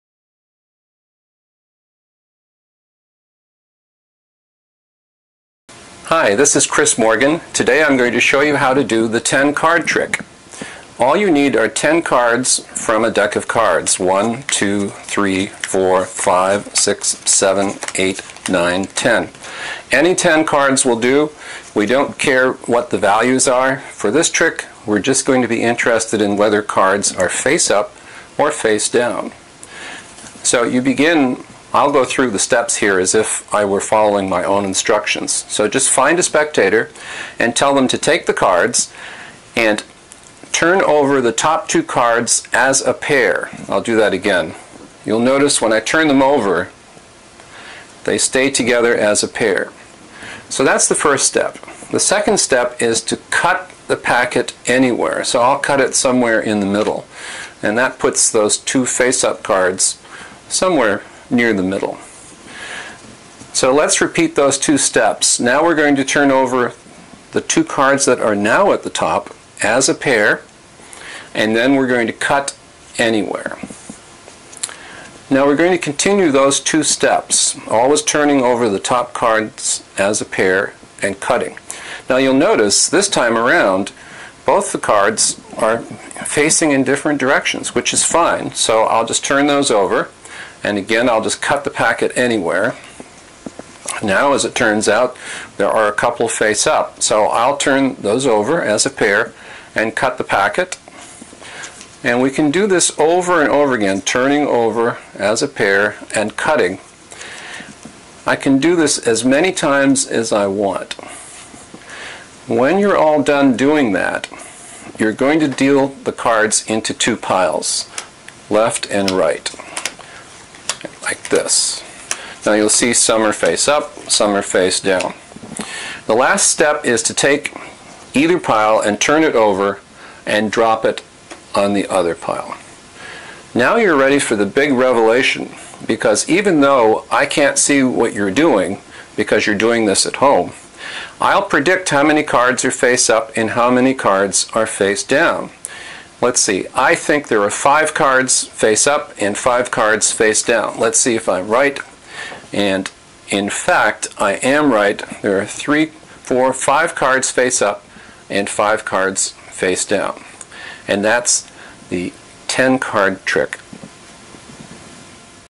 Ten Card Trick by Bob Hummer -- performance
This self-working mathematical card trick is the brainchild of the brilliant, eccentric magician, Bob Hummer, who was a friend of Martin Gardner's. The trick is featured in Gardner's book, "Mathematics, Magic and Mystery." (This version is an adaption of the original trick by Chris Morgan.) The trick relies on two mathematical principles not normally seen in card tricks: the inversion of a set, and the principle of parity.